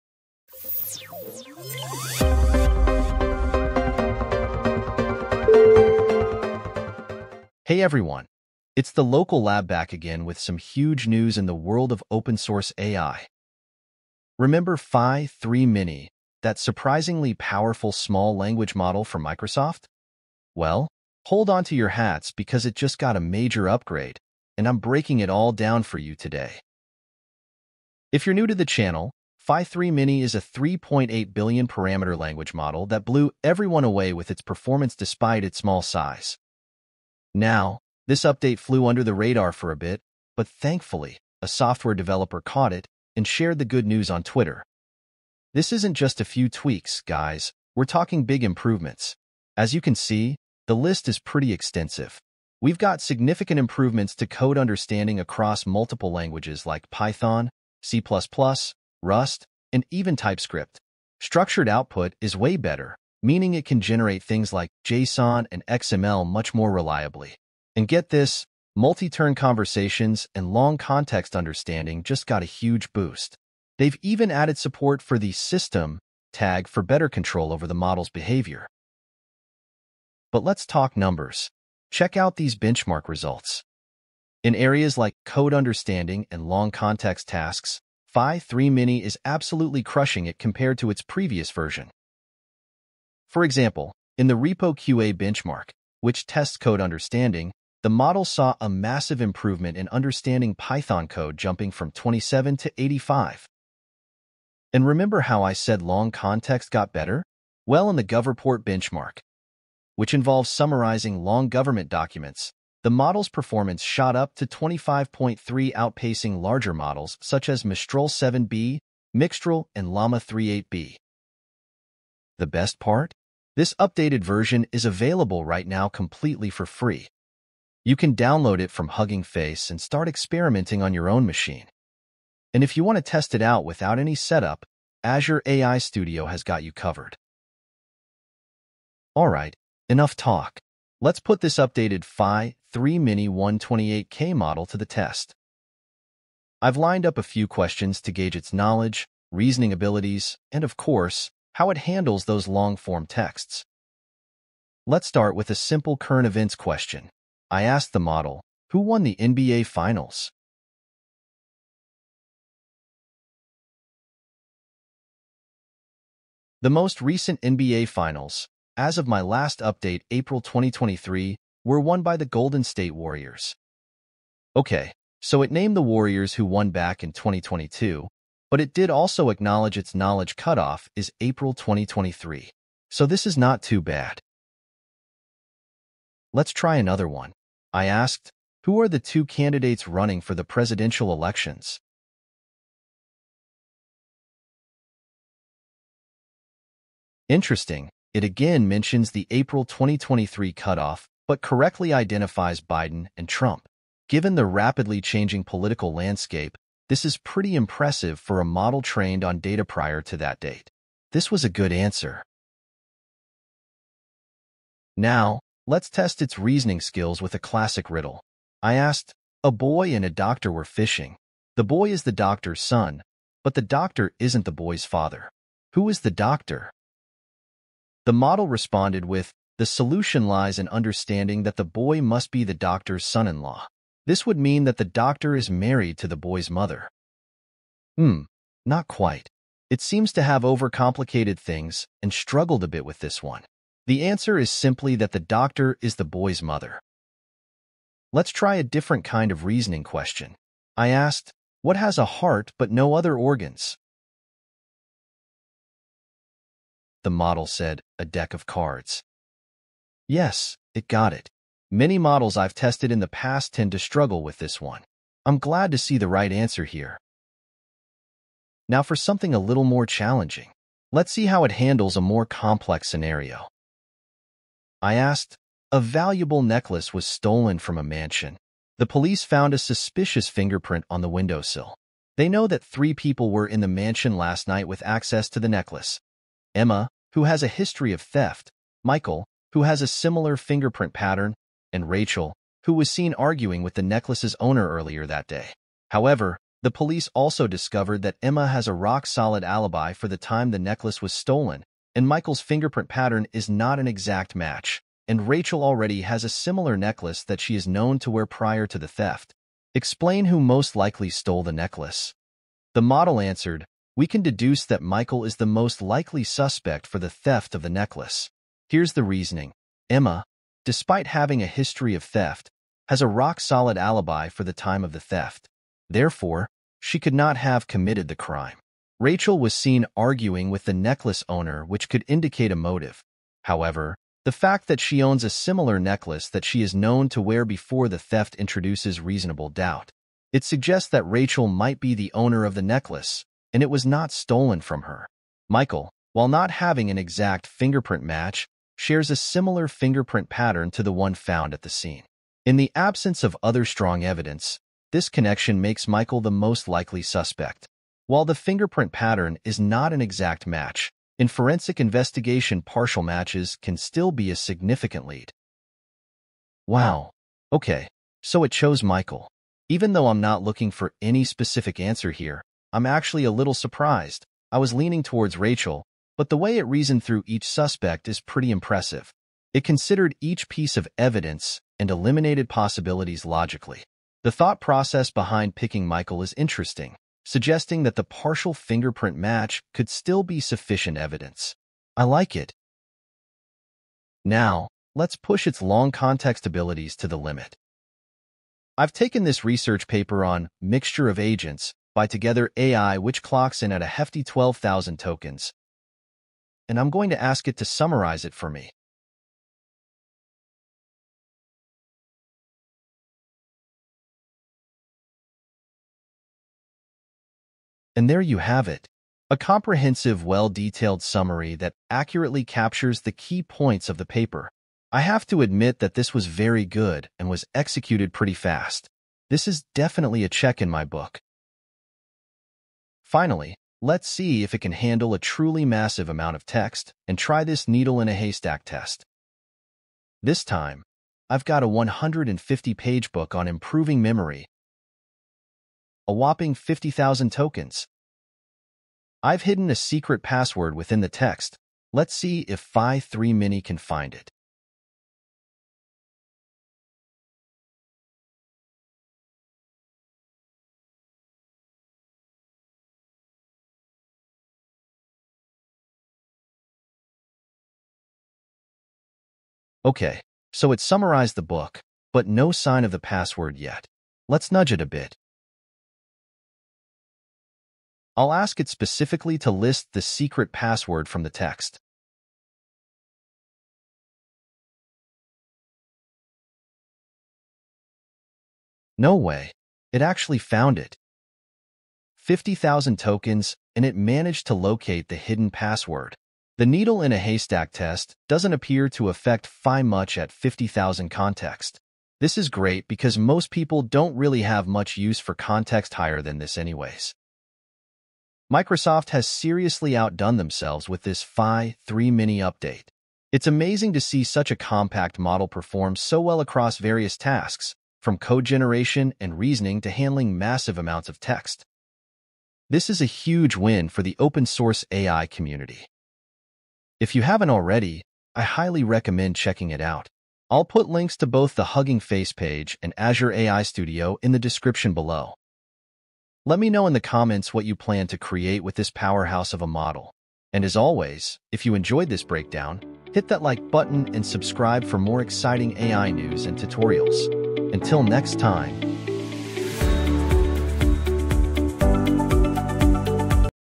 Microsoft Phi-3 Mini June 2024 Update - Beats Large Models in Long Context
Get ready for a game-changer in the world of open-source AI Microsoft's Phi-3 Mini, a 3.8 billion parameter language model, has just received a major upgrade, and we're diving into all the exciting details! In this video, we'll explore the significant improvements to code understanding, structured output, multi-turn conversations, and long context understanding.

We'll also put the updated Phi-3 Mini 128k model to the test, pushing its limits with challenging questions, reasoning exercises, and massive text summaries.

What's new in Phi-3 Mini:
Improved code understanding across multiple languages (Python, C++, Rust, TypeScript)
Enhanced structured output (JSON, XML)
Boosted multi-turn conversations and long context understanding
Benchmark results: Crushing it in code understanding and long context tasks, outperforming larger models like Mistral 7B, Mixtral, and LLama 3 8B!